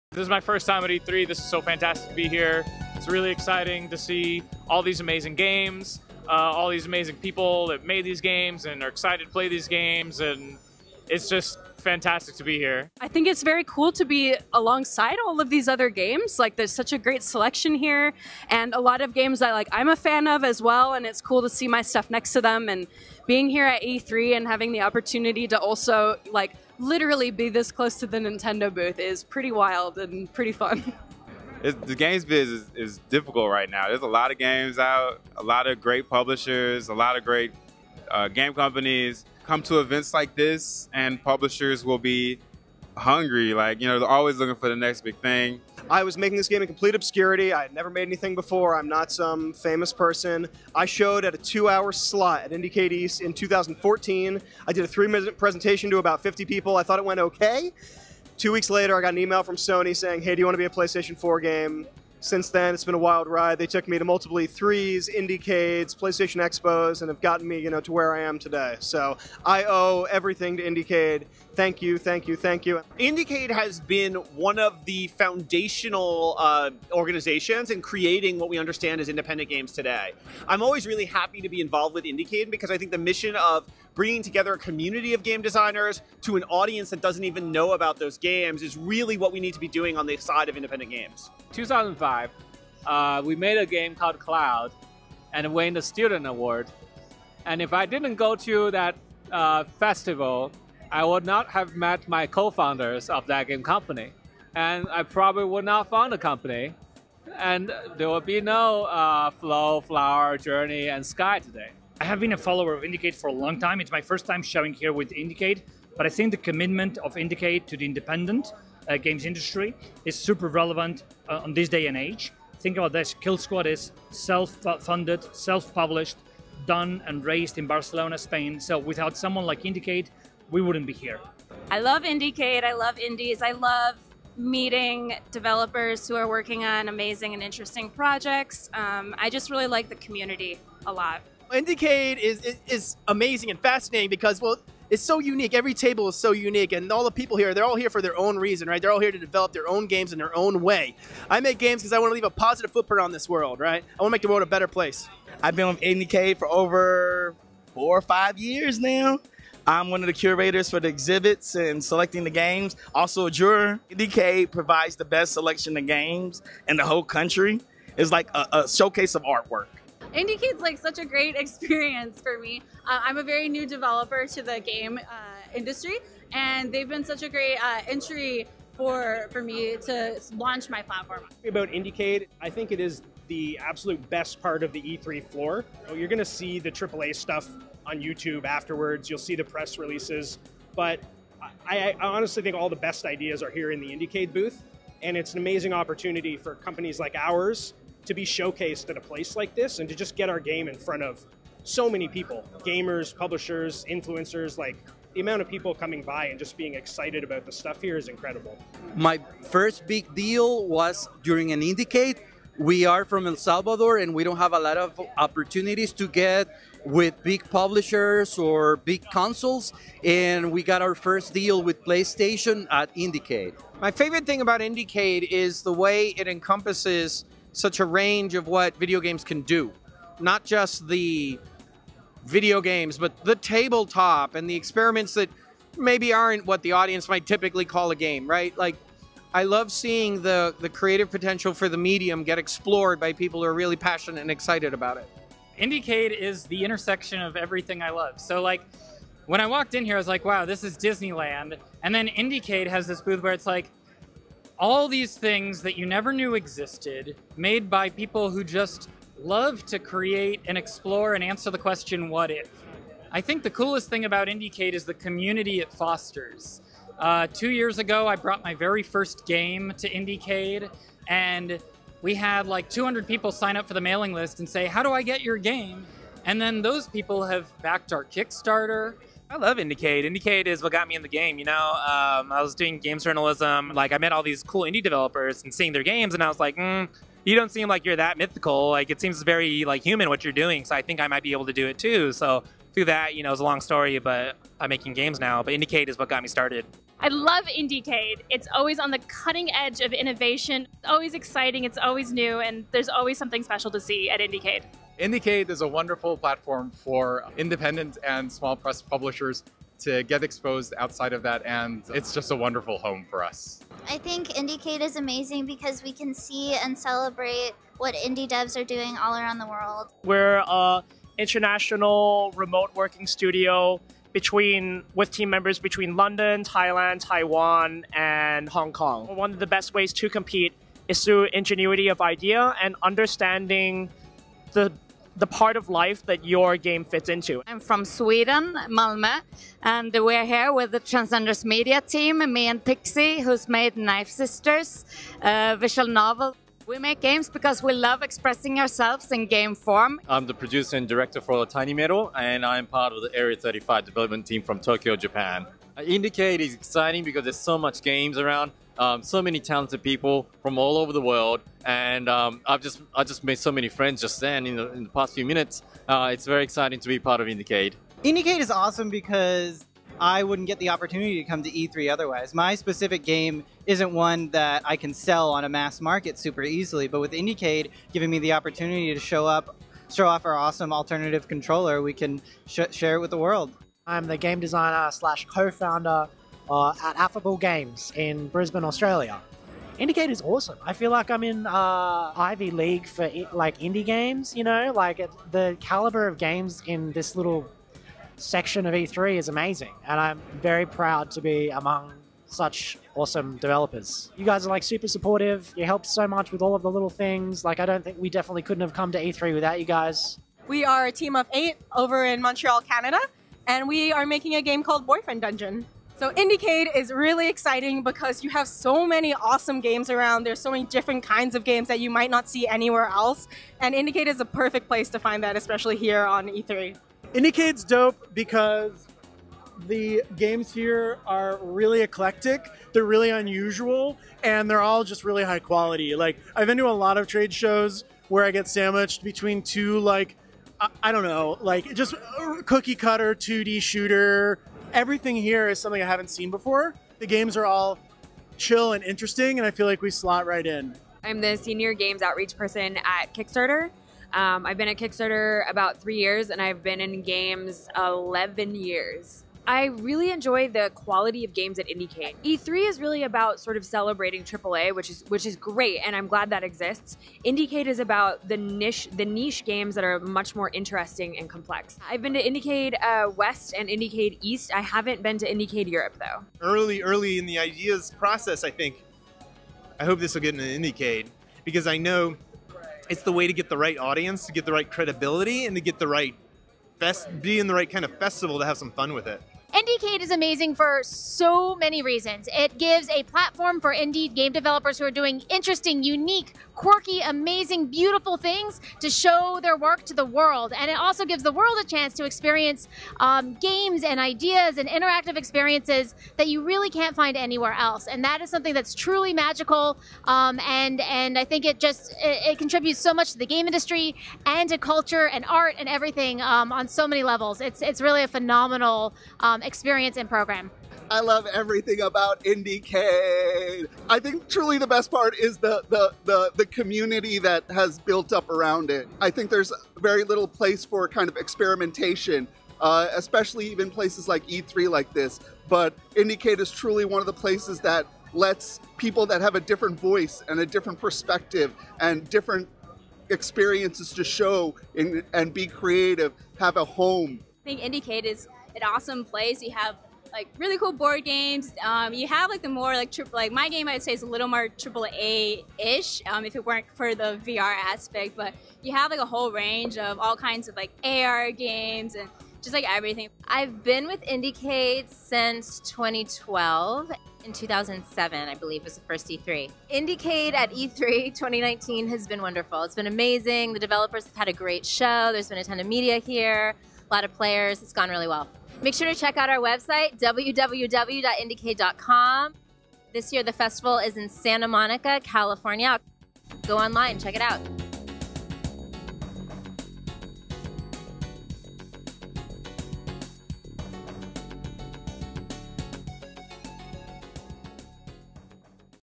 IndieCade Alumni Share Their Experiences
IndieCade Alumni at our E3 2019 Showcase share their experiences.
IndieCade Anywhere and Everywhere: October 16 — October 24
Submissions open through June 1st!